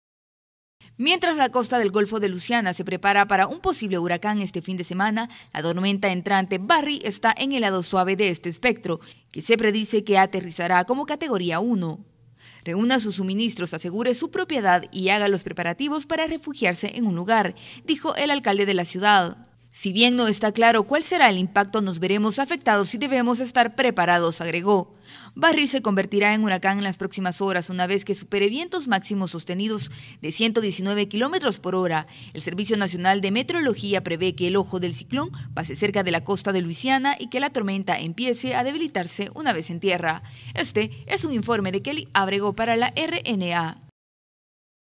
Nueva Orleans se prepara para tormenta Barry
Mientras la costa del golfo de Luisiana se prepara para un posible huracán, este fin de semana, la tormenta entrante "Barry" está en el lado manejable de ese espectro, que se predice tocara tierra como categoría uno.